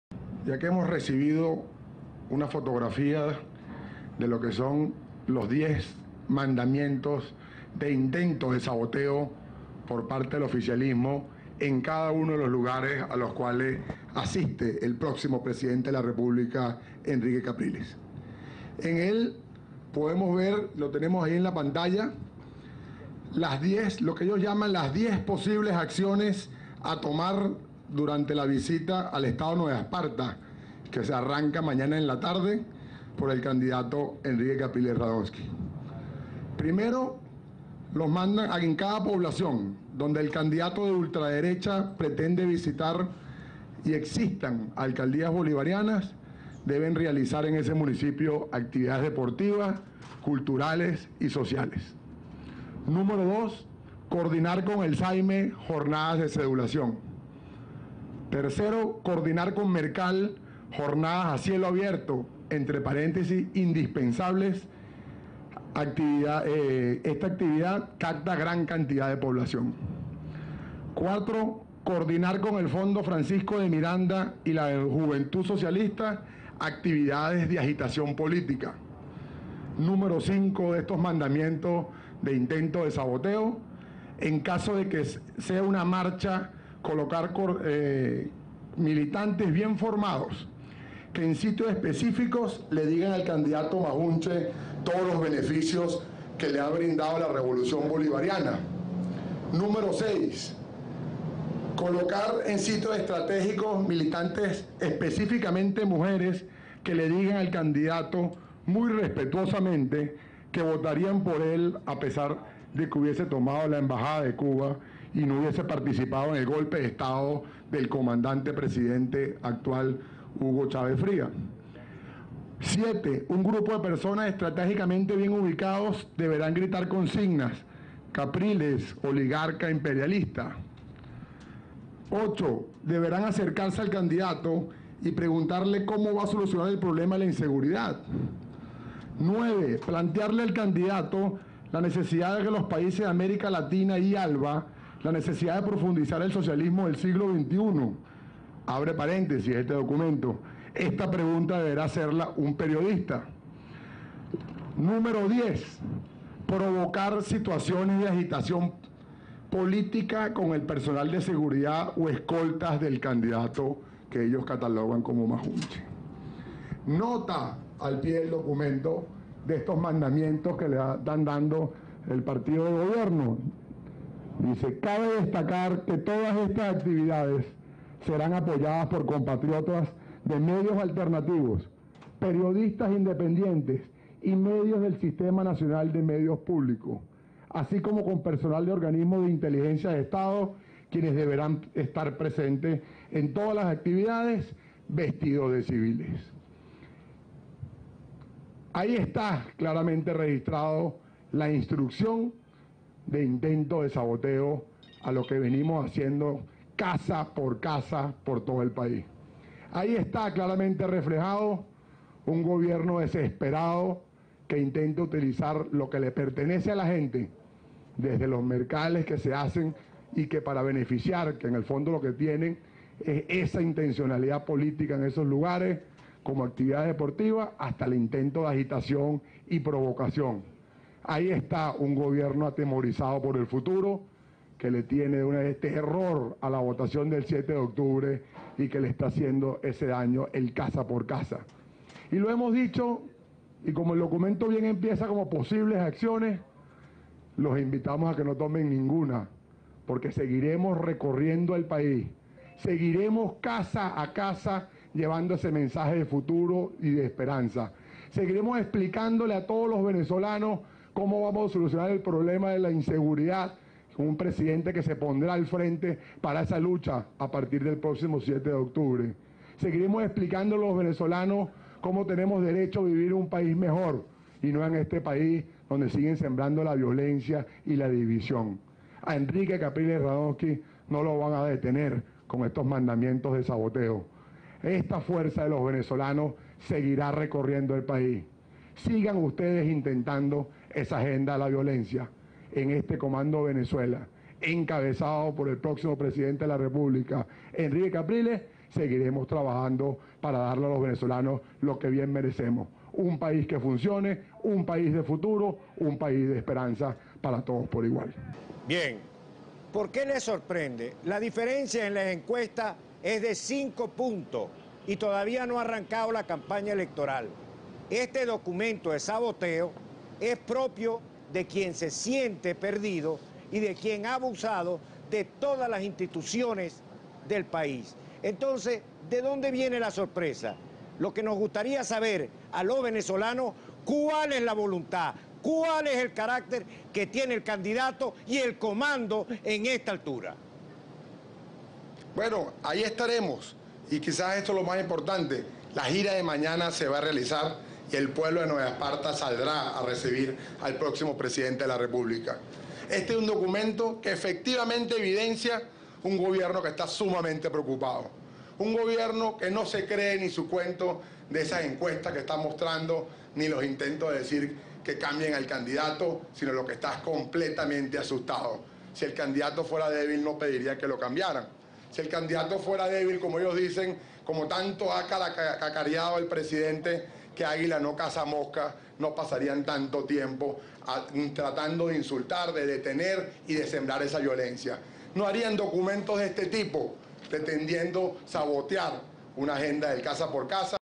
Develan plan sabotaje contra Capriles
Armando Briquet devela plan de sabotaje contra Capriles